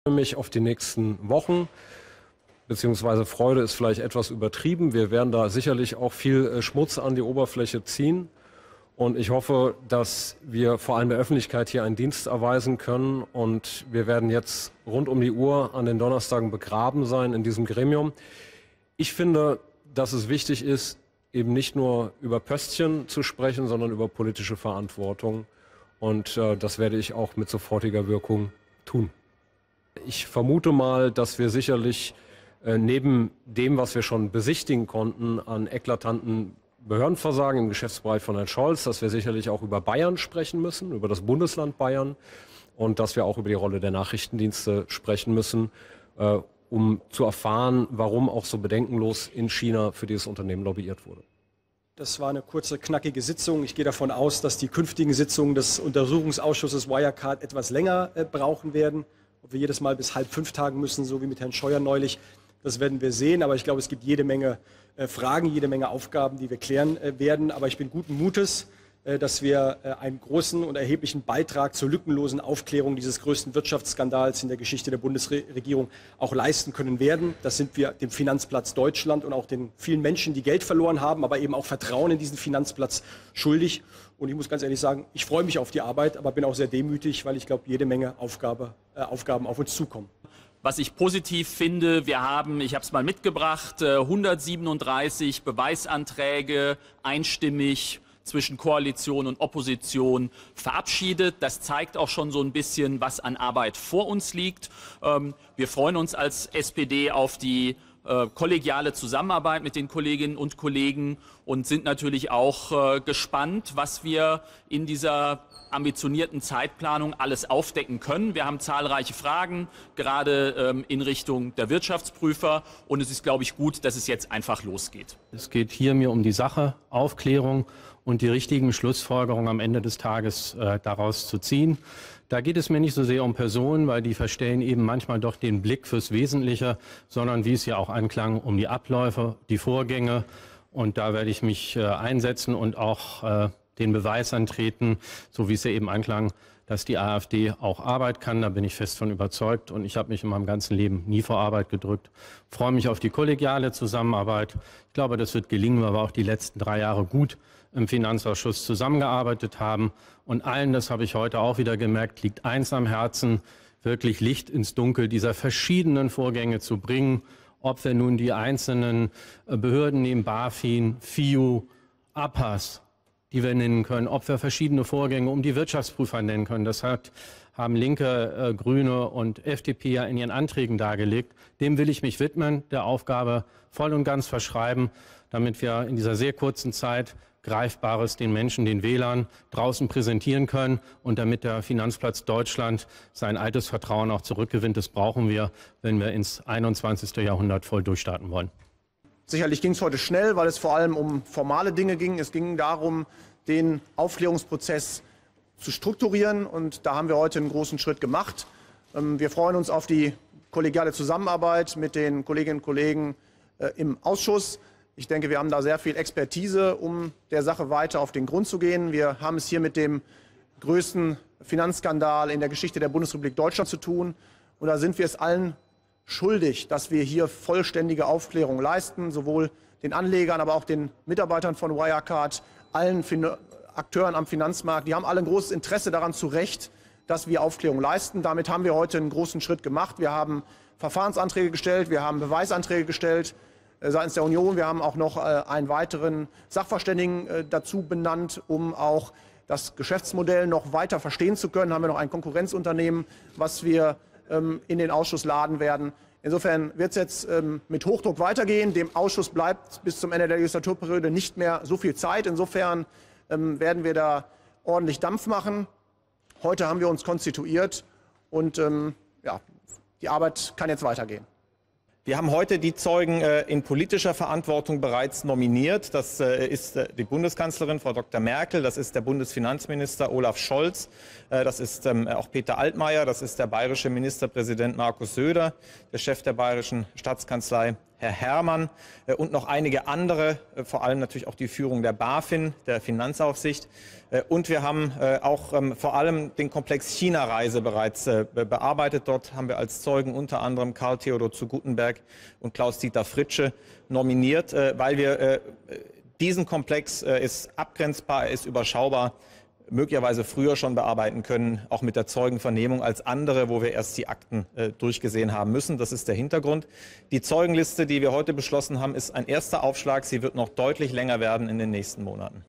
Statements der Obleute zur Konstituierung des Wirecard Untersuchungsausschusses am 08.10.20
Die Obleute im Wirecard Untersuchungsausschuss, Fabio de Masi (DIE LINKE), Danyal Bayaz (Bündnis 90/Die Grünen), Jens Zimmermann (SPD), Kay Gottschalk (AfD), Matthias Hauer (CDU) und Florian Toncar (FDP), äußern sich zur Konstituierung des Ausschusses.